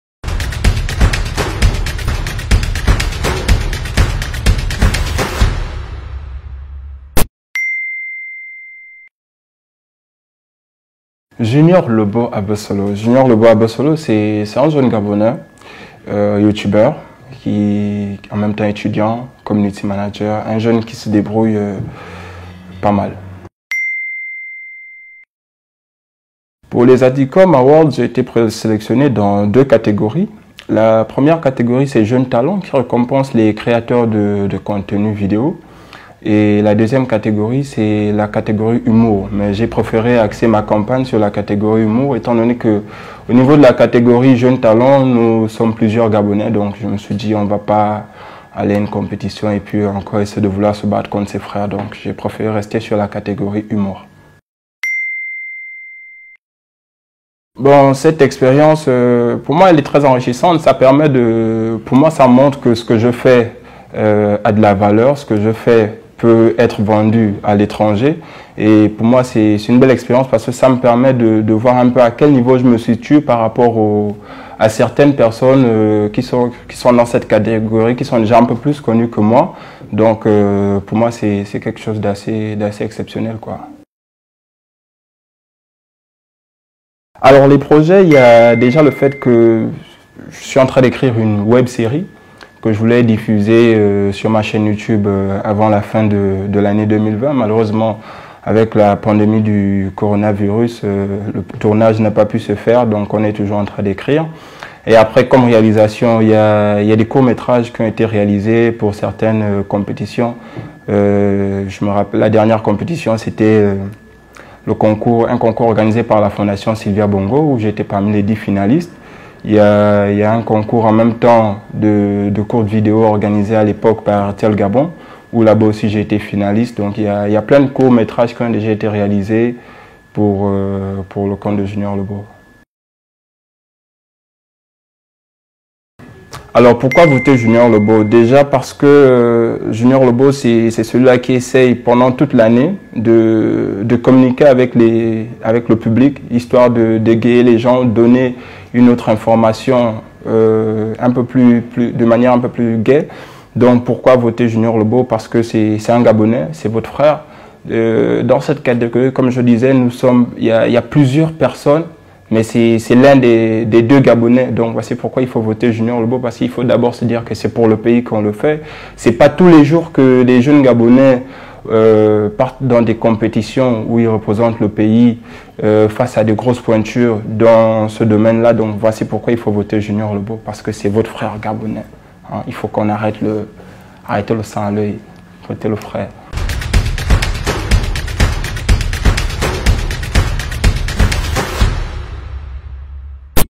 Junior Le Beau ABESSOLO nominé aux ADICOMAWARDS by Medias241
Passage dans la rubrique À VOUS LA PAROLE sur Medias241 TV à l'occasion de la 4e édition des Adicom Award